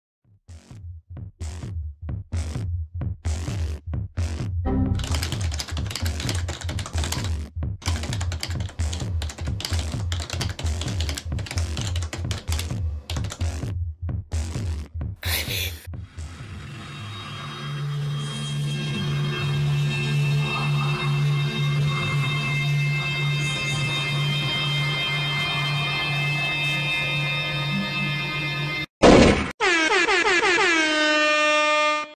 Froot Salade - 2016 24 Hours Animation Entry
Created in 24 hours by a team of animation students from Franklin High School for the 24 Hours Animation Challenge.